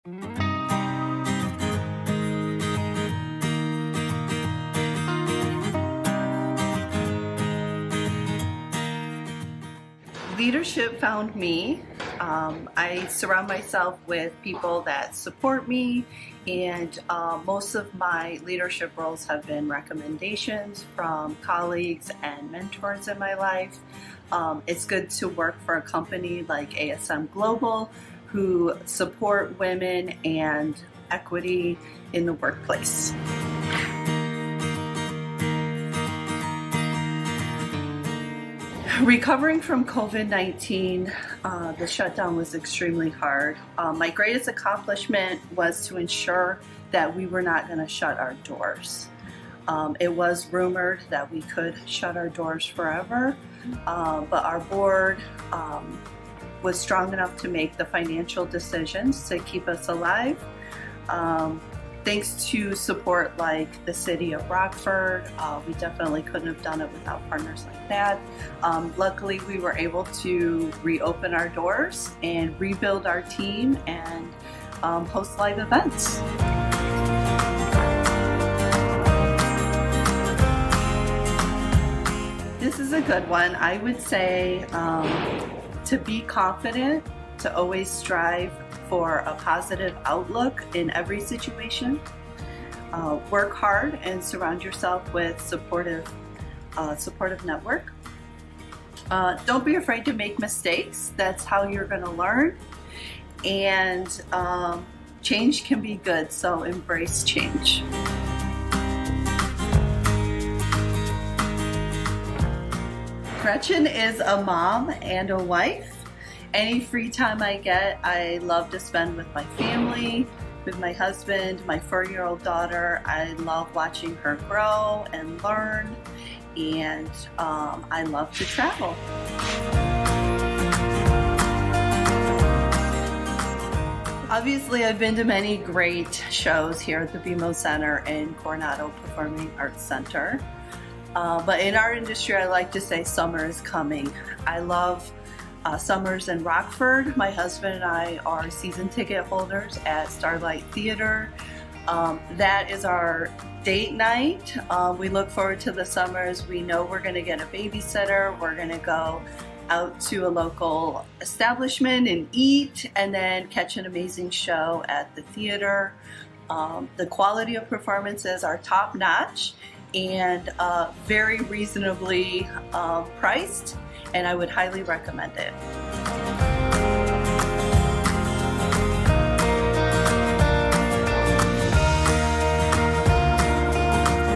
Meet The General Manager, Gretchen Gilmore
In honor of Women's History Month, we want to highlight one of the strongest and successful women we have at our venue and in the community. Meet our General Manager, Gretchen Gilmore, and get to know her as she speaks of her experience, gives advice to other women pursuing careers in leadership, and gives us a preview into her life.